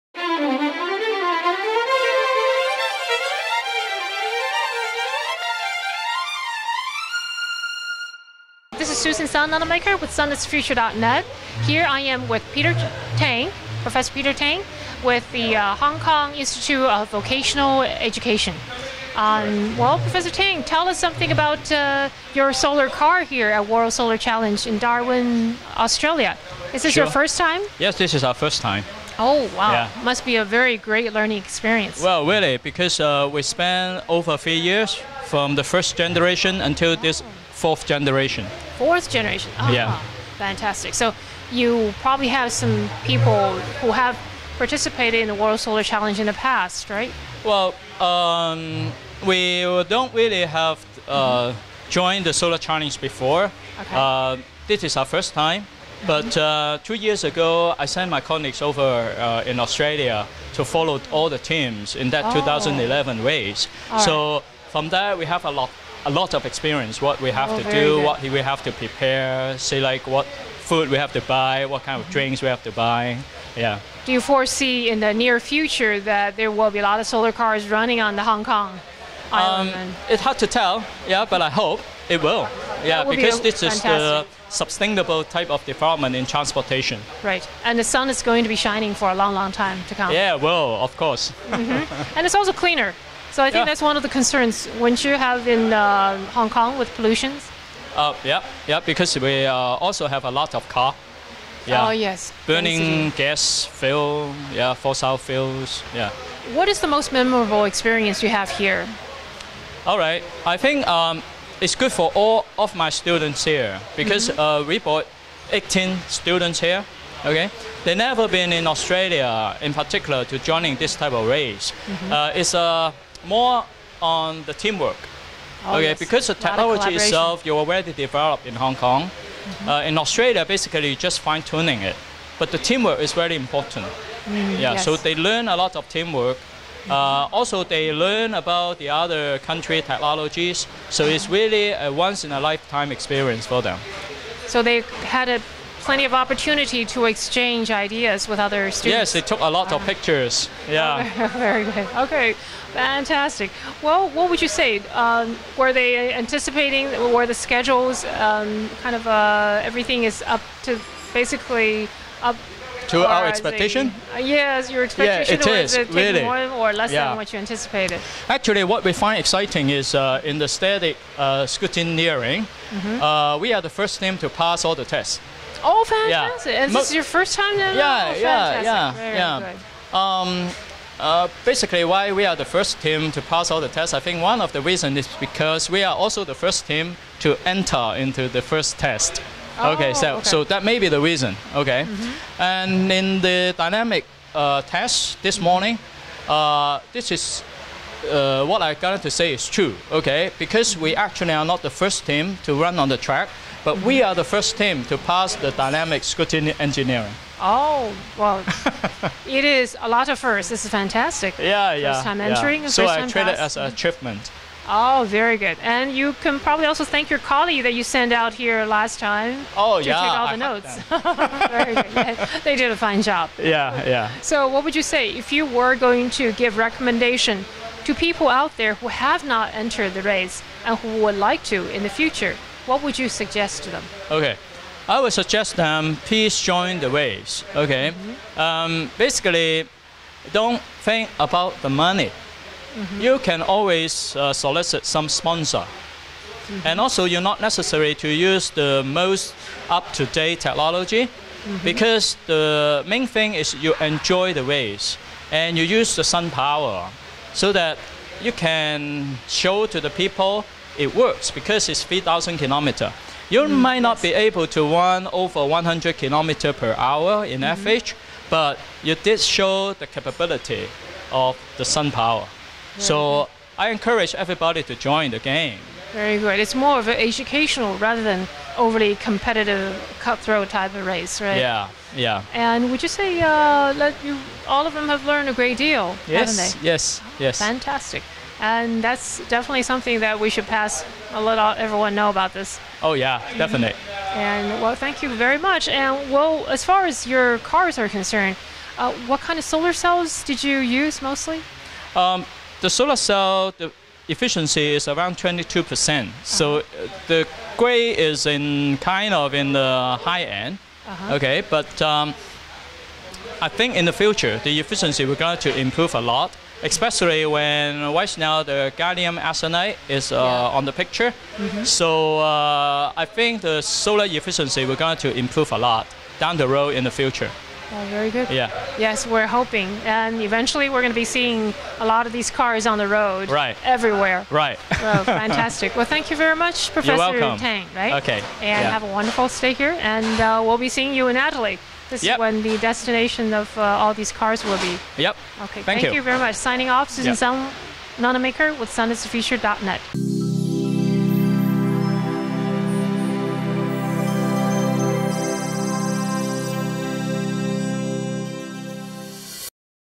Chat With Prof. Peter Tang of Hong Kong Institute of Vocational Education on SOPHIE IV at WSC2013
This is an interview with Prof. Peter Tang of Hong Kong Institute of Vocational Education about IVE Solar Car Team's experience in building SOPHIE IV and participating in World Solar Challenge 2013 at Darwin, Australia, the day before the race begins (Oct. 6, 2013). Prof. Tang emphasized the importance of participation, collaboration, and sharing/promoting the solar energy experience. This is the first time for Hong Kong Institute of Vocation Education to participate in World Solar Challenge and their SOPHIE IV passed the scrutineering process beautifully! Great job for IVE Solar Car Team!  The interview was conducted and uploaded by sunisthefuture-Susan Sun Nunamaker and filmed by Michael Nunamaker

For more photos and videos of all solar cars participating in World Solar Challenge (WSC) 2013, please visit sites below:

sunisthefuture-Susan Sun Nunamaker